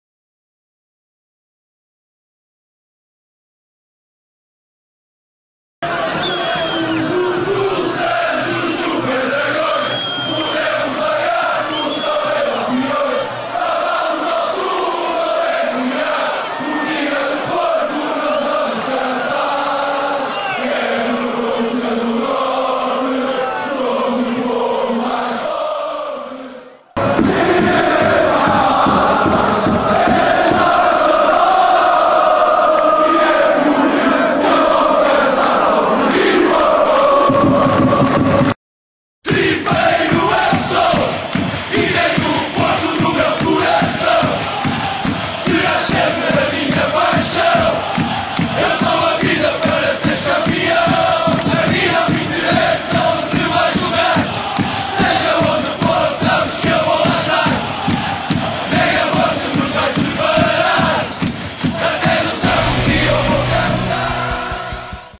Marc Janko papou o Super Maxi
Este duelo passou-se no derby benfica-porto que acabou com a vitoria do GRANDE PORTO!!

Cliquem em "Gosto"